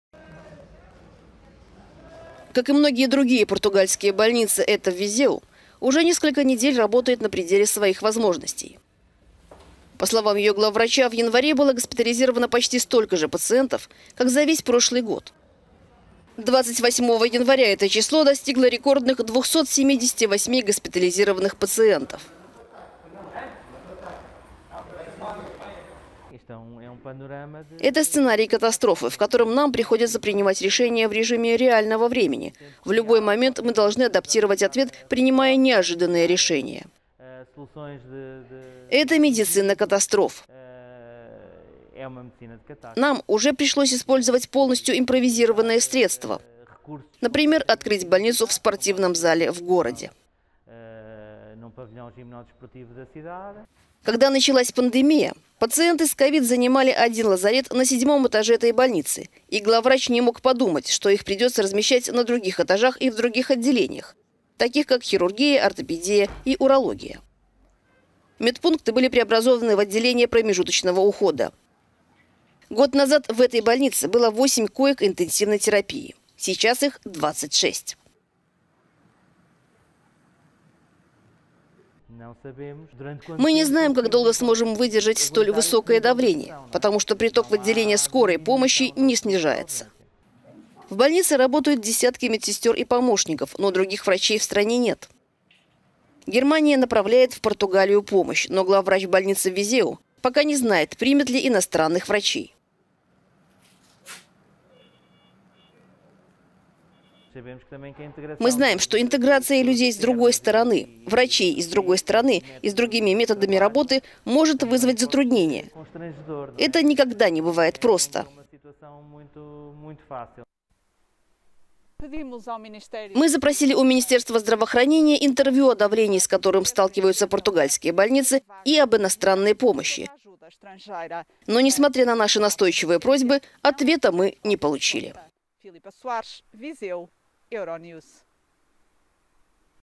Больницы Португалии на пределе возможностей
Как и многие другие португальские больницы, эта в Визеу уже несколько недель работает на пределе своих возможностей. По словам ее главврача, в январе было госпитализировано почти столько же пациентов, как за весь прошлый год.